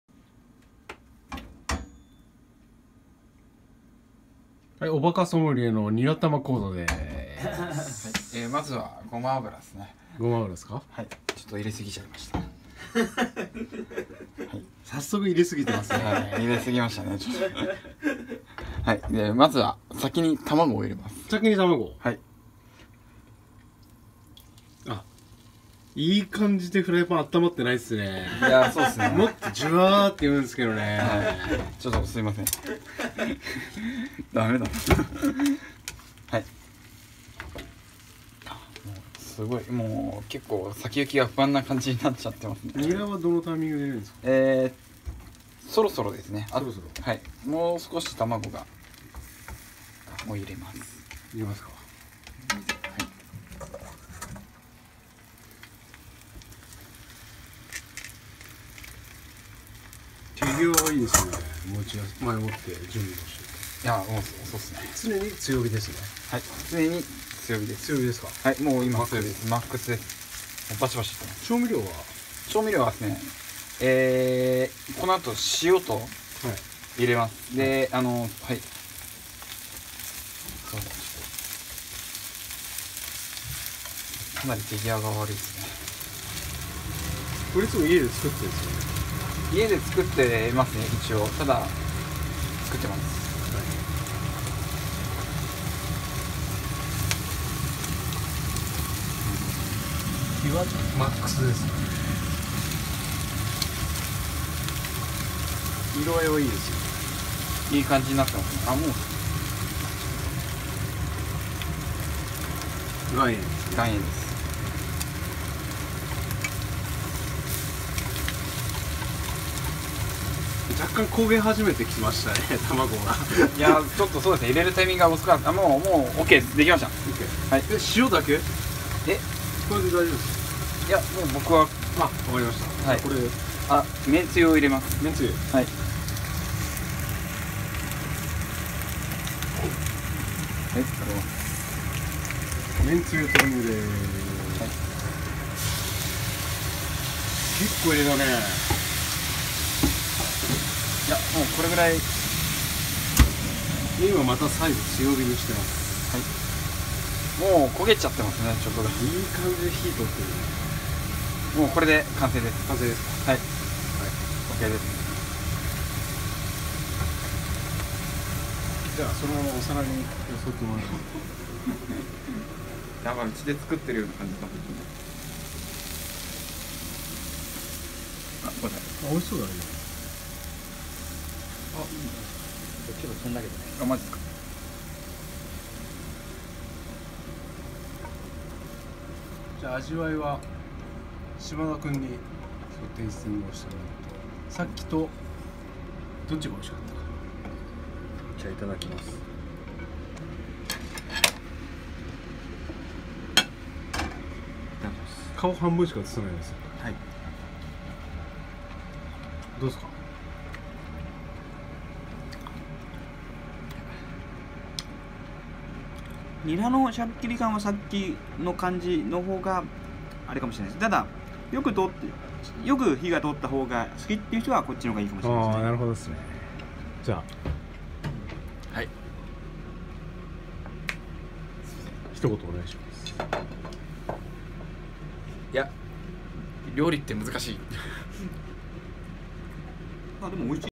おバカソムリエ 鈴木のニラ玉講座！
普段一人暮らしで、自炊は基本ニラ玉の
鈴木ソムリエに、ニラ玉を作ってもらいました❗️